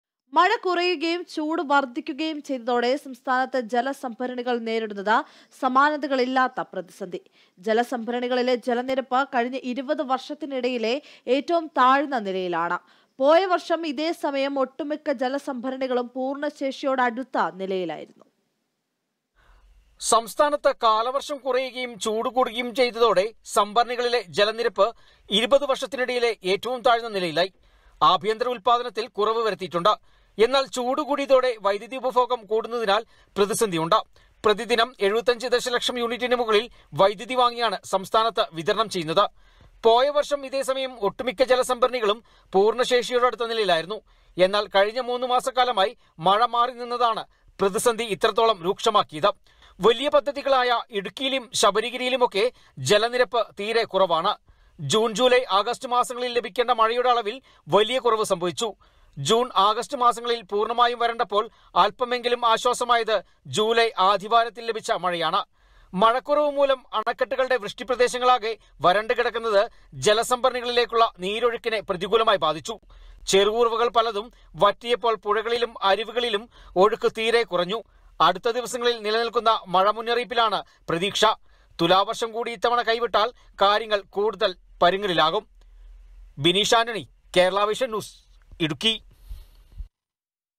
പ്രതിസന്ധിയില്‍ സംസ്ഥാനത്തെ ജലസംഭരണികള്‍; ജലനിരപ്പ് 20 വര്‍ഷത്തിനിടയിലെ ഏറ്റവും താഴ്ന്ന നിലയില്‍
മഴ കുറയുകയും ചൂട് വര്‍ധിക്കുകയും ചെയ്തതോടെ സംസ്ഥാനത്തെ ജല സംഭരണികള്‍ നേരിടുന്നത് സമാനതകളില്ലാത്ത പ്രതിസന്ധി.ജലസംഭരണികളിലെ ജലനിരപ്പ് കഴിഞ്ഞ 20 വര്‍ഷത്തിനിടയിലെ ഏറ്റവും താഴ്ന്ന നിലയിലാണ്.പോയ വര്‍ഷം ഇതേ സമയം ഒട്ടു മിക്ക ജലസംഭരണികളും പൂര്‍ണ്ണ ശേഷിയോടടുത്ത നിലയിലായിരുന്നു.

About COA